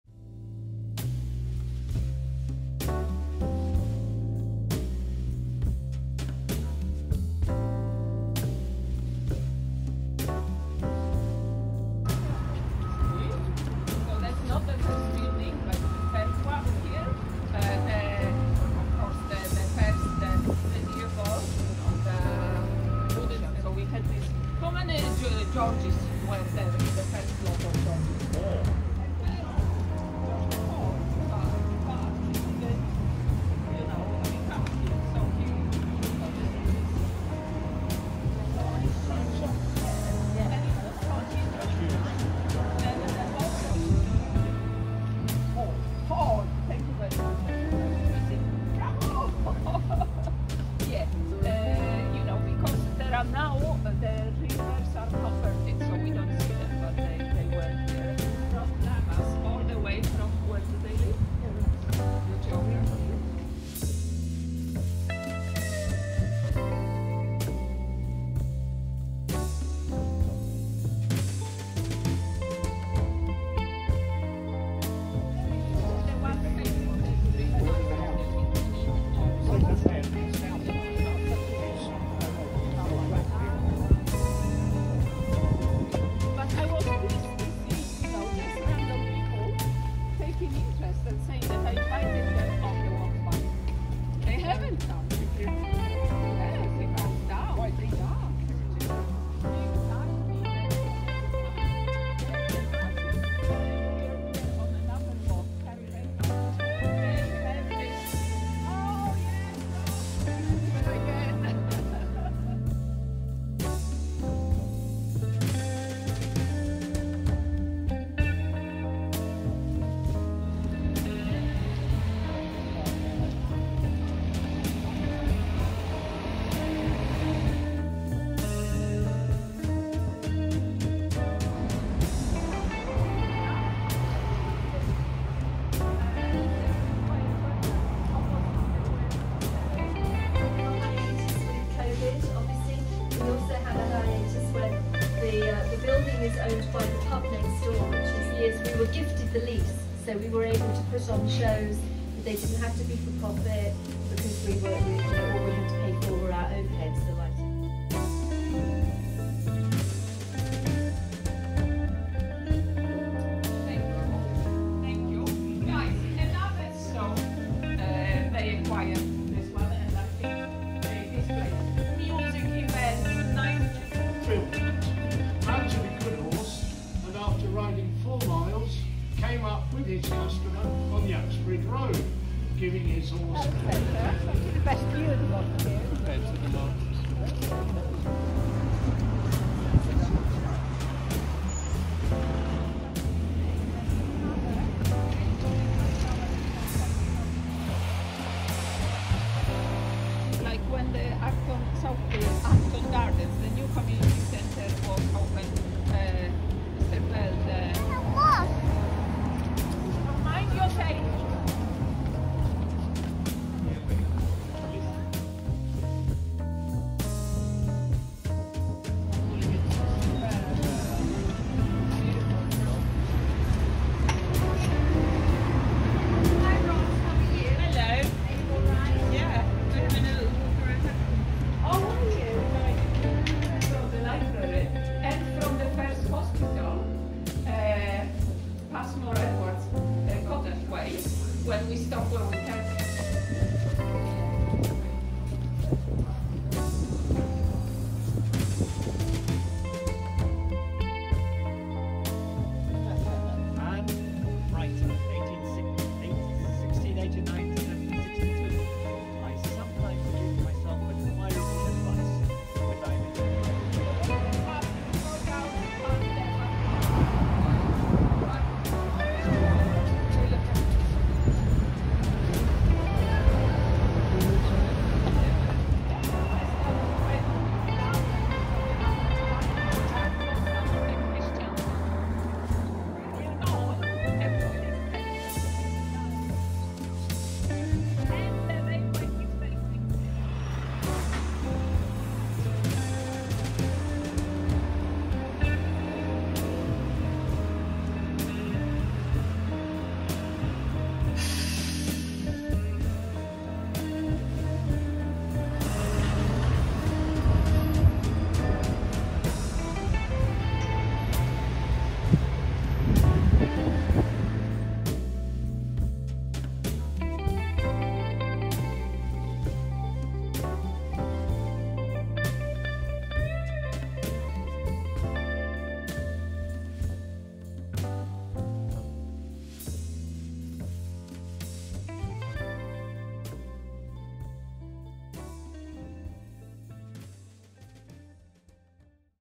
Acton Together Walk 1/6: Around Acton High Street
See what our first of six ACTON TOGETHER walks was like! We met outside St Mary's Church and walked through the streets around the High Street stepping into a park, an art gallery, a former coaching inn and the local community cinema housed in the former John Passmore Edwards library.

The sun was on our side and the fresh spring foliage made a great backdrop for pictures and video clips.

How lucky we are to have dear Truus in our group! Not only did she record us on the way but also gave her time to learn new skills after the walk to make it all into this video!